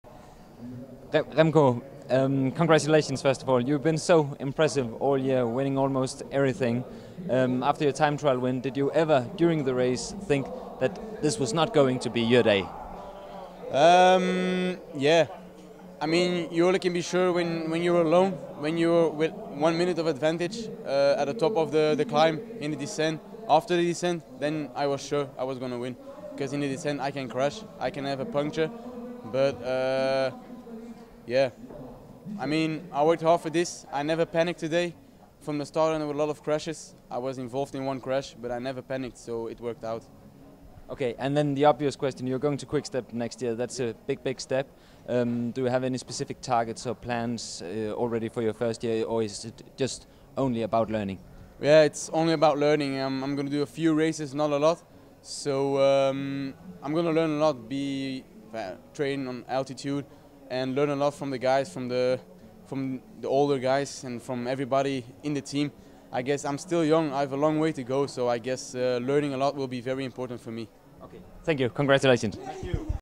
VM 2018: Remco Evenepoel om sit dobbelte verdensmesterskab
Det belgiske stortalent, Remco Evenepoel, vandt torsdag sit andet verdensmesterskab på en uge. Belgieren kørte konkurrenterne midtover.